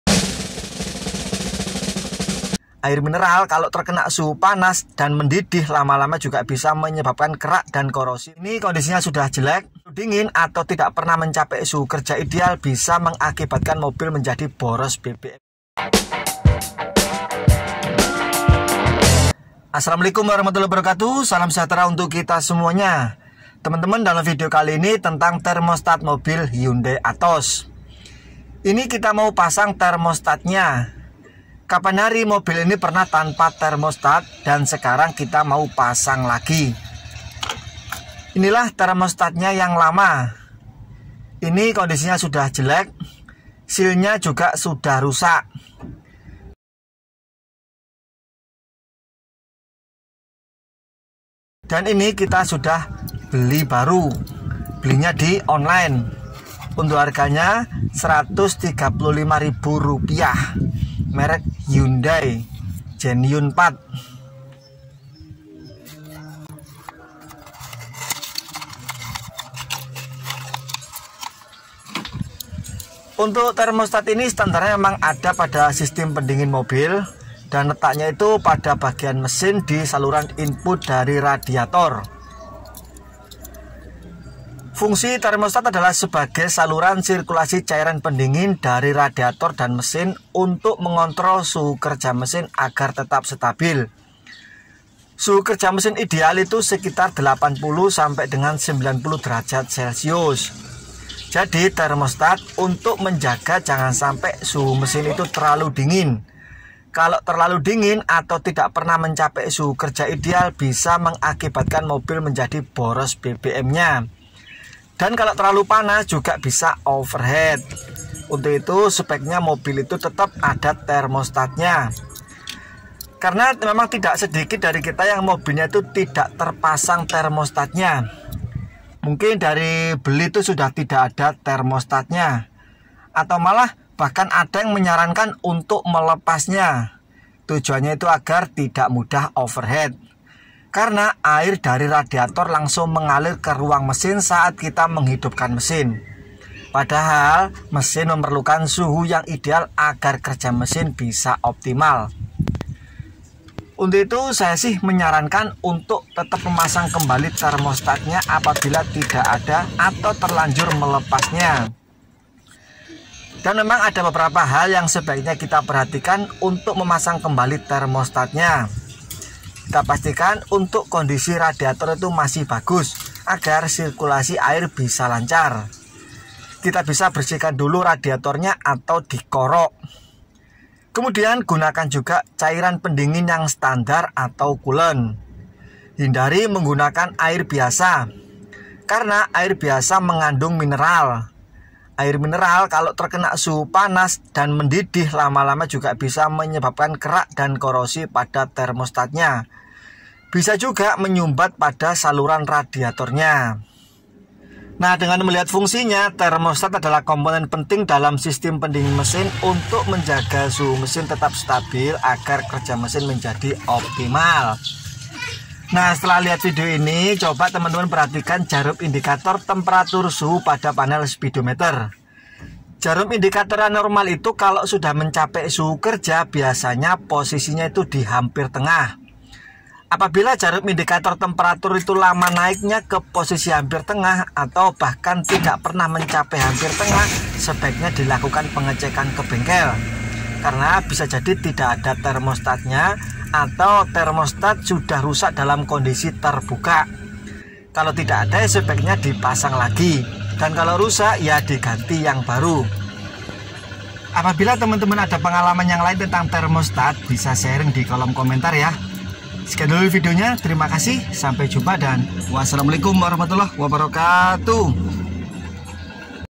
MEMASANG THERMOSTAT MOBIL HYUNDAI ATOZ
Video tentang memasang kembali thermostat mobil hyundai atoz 1000cc,karena sebelumnya thermostat dilepas kaena kondisi audah tidak layak lagi,ketahuan kondisi sudah tidak bagis kerika rurun mesin akibat dari timing belt putis dan sekarang sudah terpasang,radiator juga sudah baru dan juga menggunakan air coolant
Tanpa thermostas efeknya bisa membuat mesin lama mencapai suhu kerja ideal,akibatnya bbm menjadi boros,temperatur suhu juga terkadang tidal stabil menjadi rendqh kerika beroendara di kondiso hawa dingin
Bagi teman2 yang mobilnya tidak menggunakan thermostat saran saya untuk measang kembali thermostatnya,namun sebelum memasqng sebaiknya kita perhatikan kondisi radiator,setidaknya  sudah dibersihkan agar sirkulasi air bisa lancar,selang radiator juga diperiksa kondisinya,dan usahakan juha menggunakan cairan coolant sebagai standaenua,ketika kita menggunakan air sumur atau semacamnya dalam jangka wqktu tertentu bisa menyebabkan karat dan korosi,karena rheemostat berbahan dari besi,selain itu juga bisa menyebabkan kerak pada radiator,akbibatnya biaia menghambat aliran cairan pendingin mobil